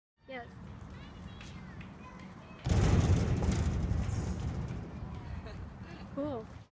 Teen dressed as Spider-Man amazes with his parkour tricks
A Colerain Township teen is showing off some amazing web-slinging acrobatic skills as he takes to the streets in a Spider-Man costume.